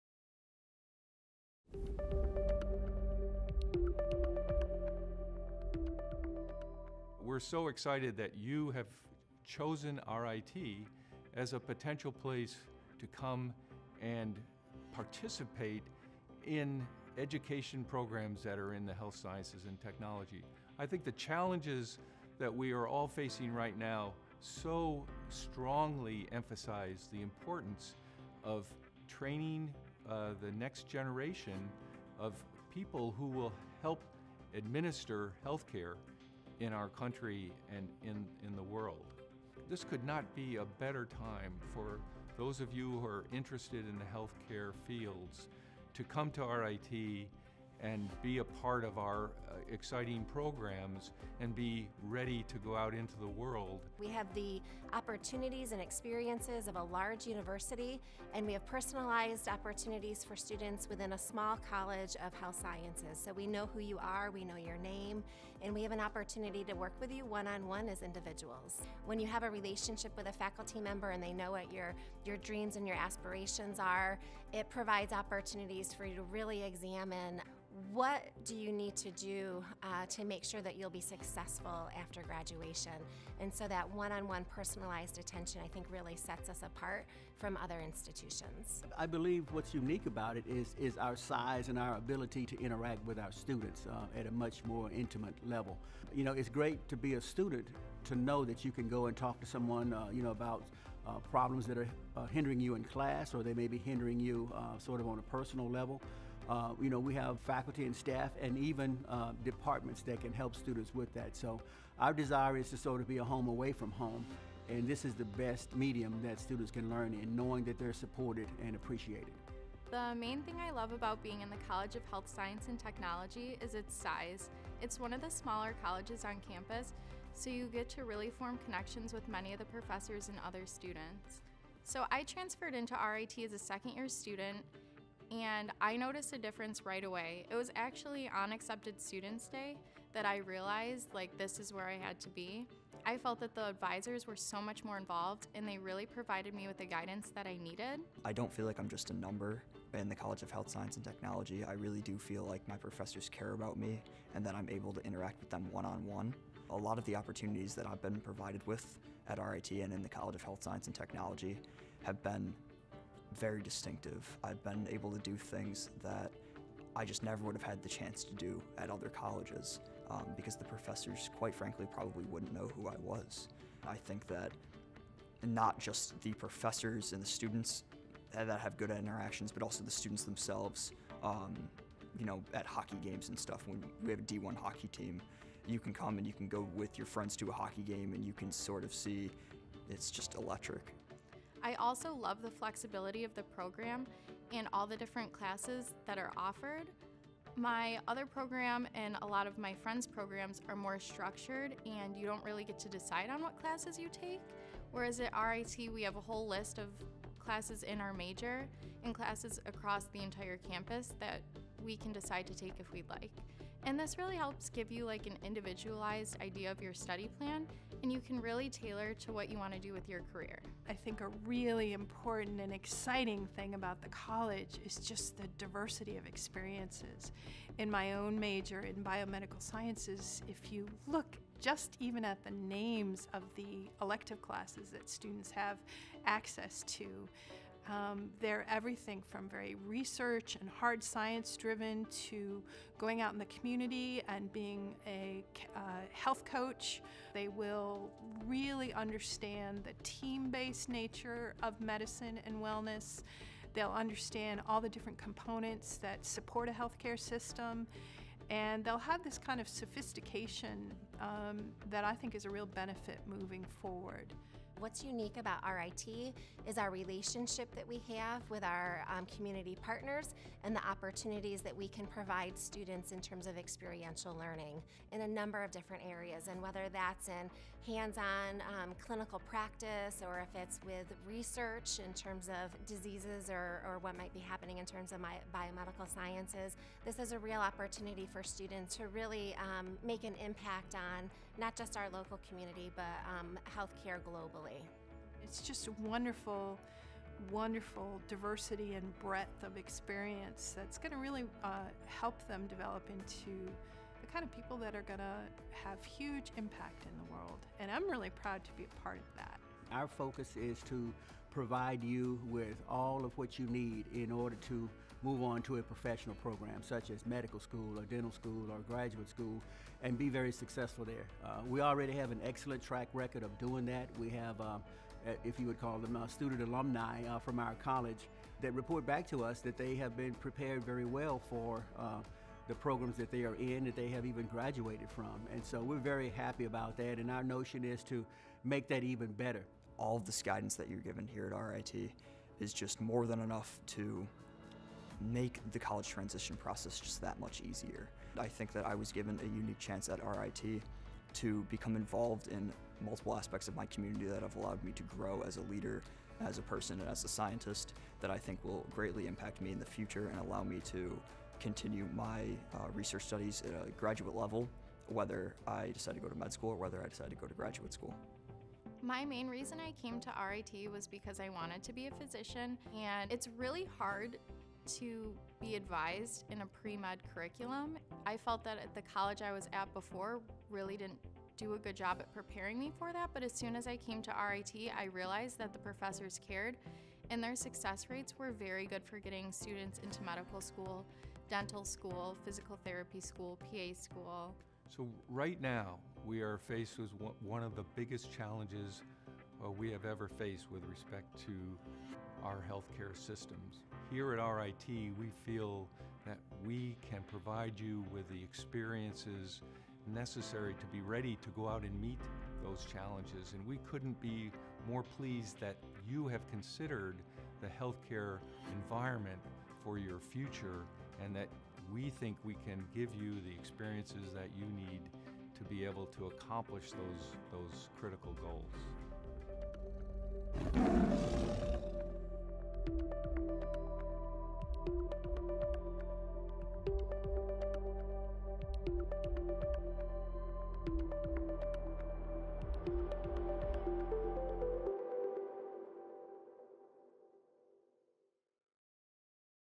See Why You Belong in RIT's College of Health Sciences and Technology
Get an in-depth look at the College of Health Sciences and Technology from our faculty and students. Let us share with you the personalized attention you’ll receive from professors who will get to know you and your professional aspirations. And, get to know the college’s culture and its friendly, supportive environment—all designed to prepare you for success.